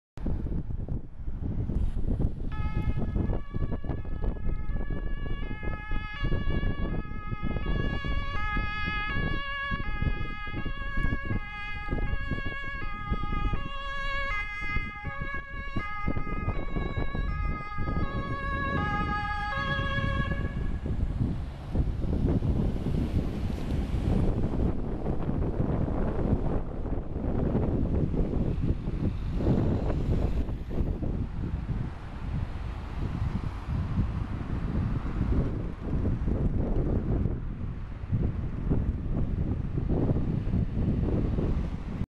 HD - RTW (GARD - Hamburg) auf Einsatzfahrt - Hamburg Stellingen
Vermutlich Reserve RTW der Gemeinnützige Ambulanz und Rettungsdienst - Hamburg auf Einsatzfahrt in Hamburg Stellingen.

Mai 2011